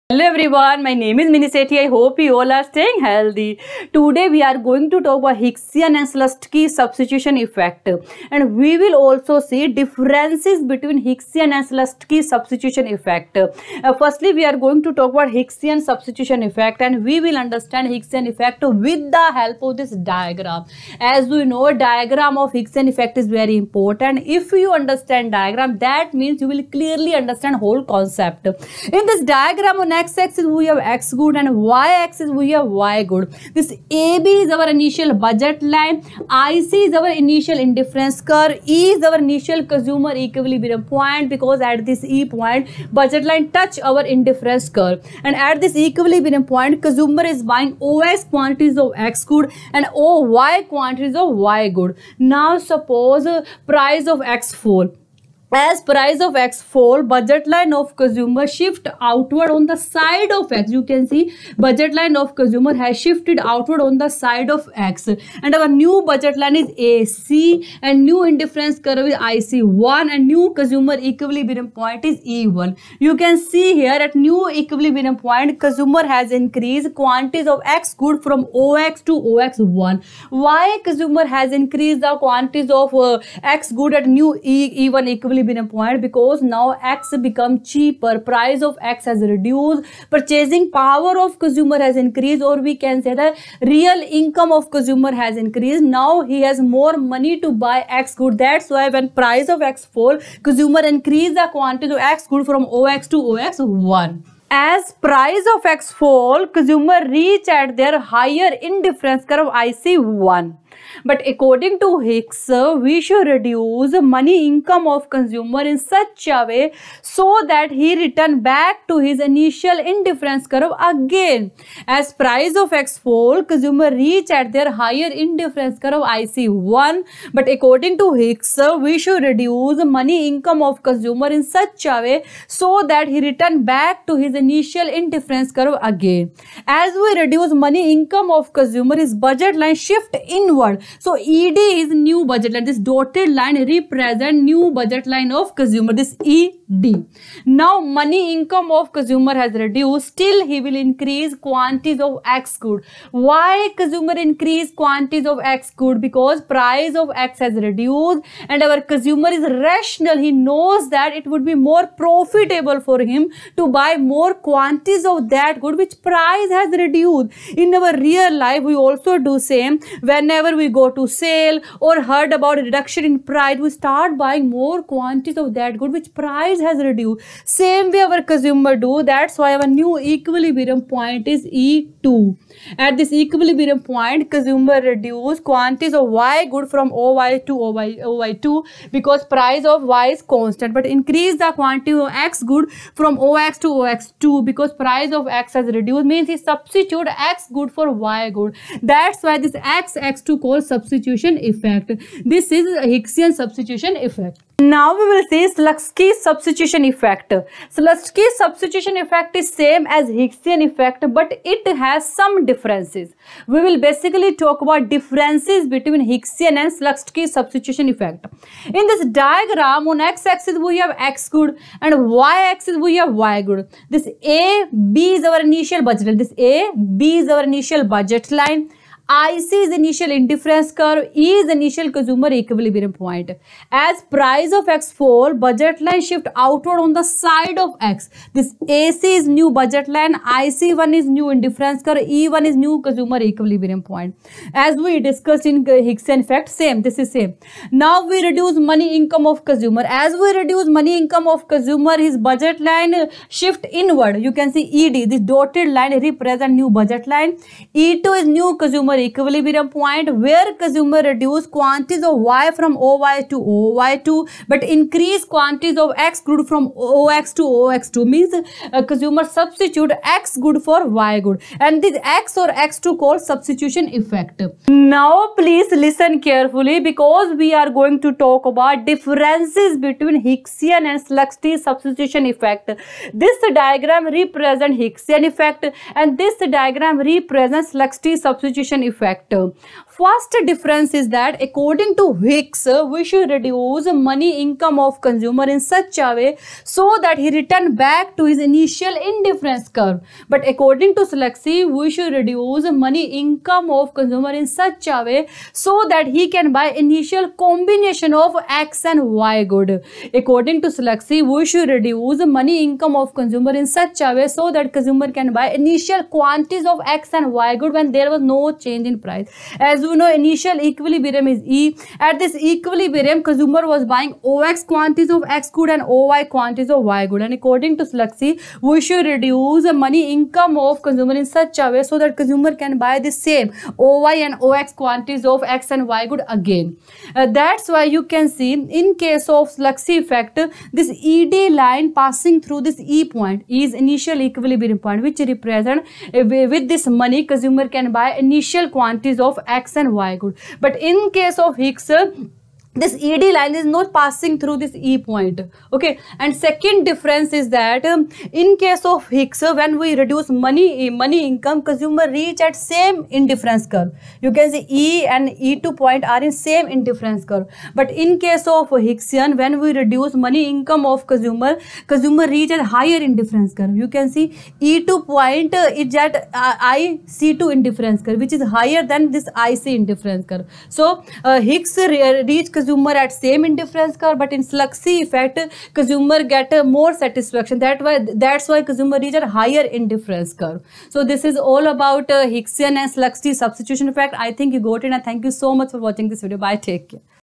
Hicksian & Slutsky Substitution Effect
This video describes about Hicksian & Slutsky
Substitution Effect and differences between them